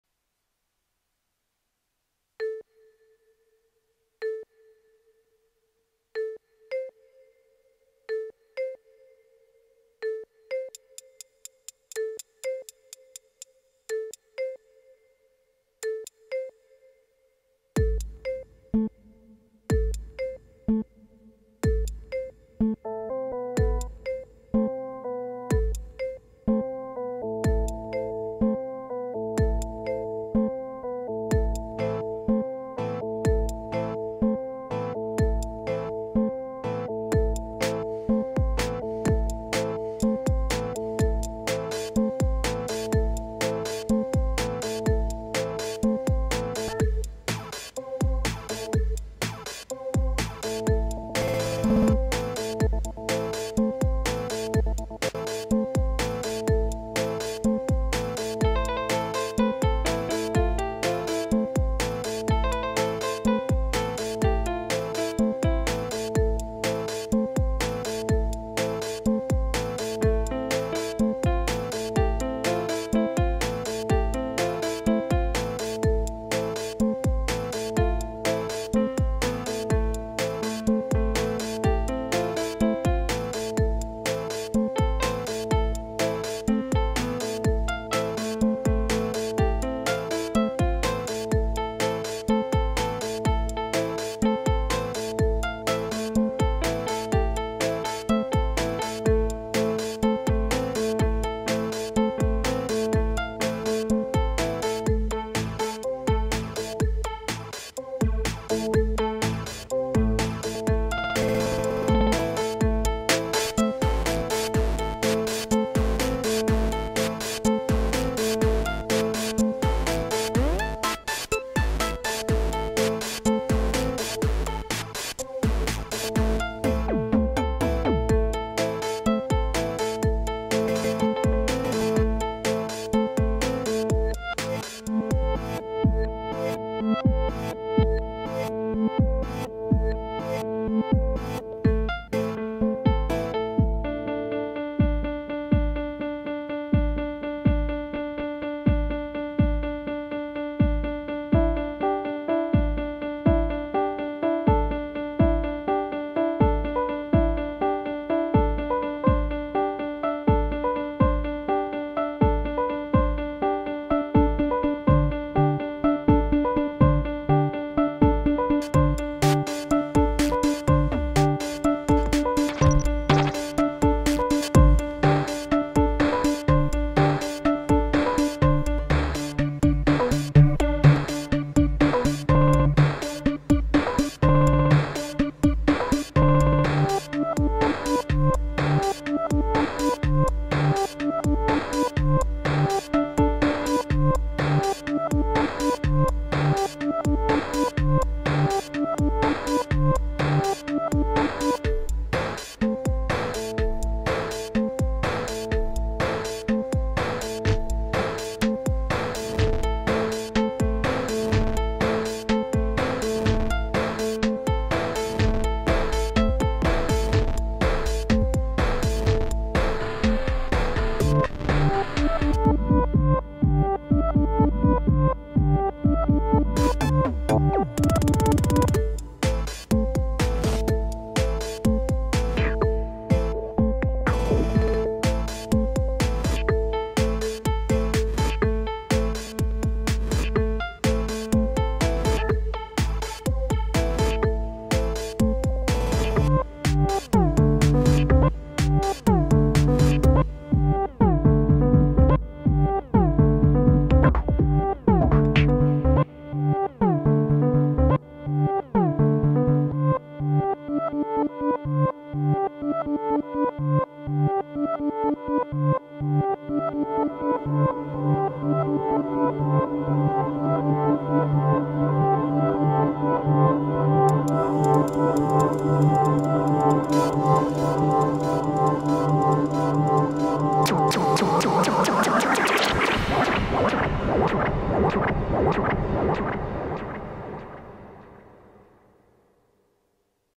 Weekly2022 #008 HWJ “Improv-from-scratch” (sorry for camera angle!)
Hardware Jams Weekend Challenge:

IMPROV FROM SCRATCH!

124bpm
Aminor, because even when using scale-constrained-grid-based sequencing, black keys make my brain stop working

(Sorry for the angle, I ended up doing this at work!)

PO-33
PO-133
PO-12